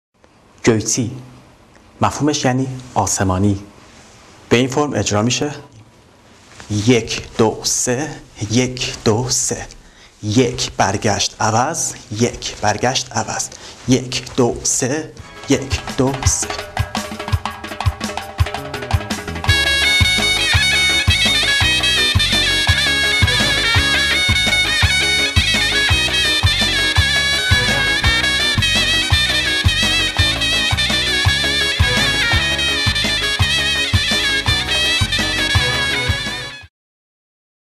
آموزش رقص آذری: 9 ( گویچی ) توسط " یاشار ایرانی " بنیان گذار و مربی گروه اوتلار
آموزش رقص آذربایجانی: (9) حرکت ( گویچی ) توسط " یاشار ایرانی " بنیان گذار و مربی گروه اوتلار

Learn Azeri Lezgi Dance OtLAR